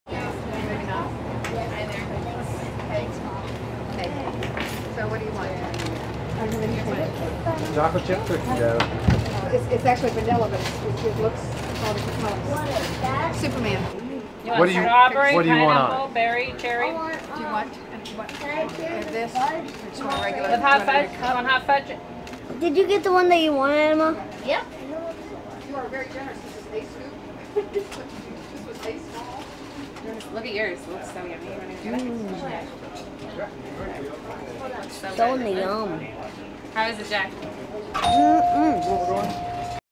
John Edwards - Ice Cream in Perry, Iowa
John, Elizabeth, Jack and Emma Claire Edwards stop for ice cream in Perry, Iowa on August 13, 2007.

The Edwards family is in Iowa for the weeklong Fighting For One America bus tour.  The tour takes the Edwards family from the Iowa State Fair to town hall meetings and community gatherings across the state, culminating in the Democratic Presidential Debate on August 19 at Drake University.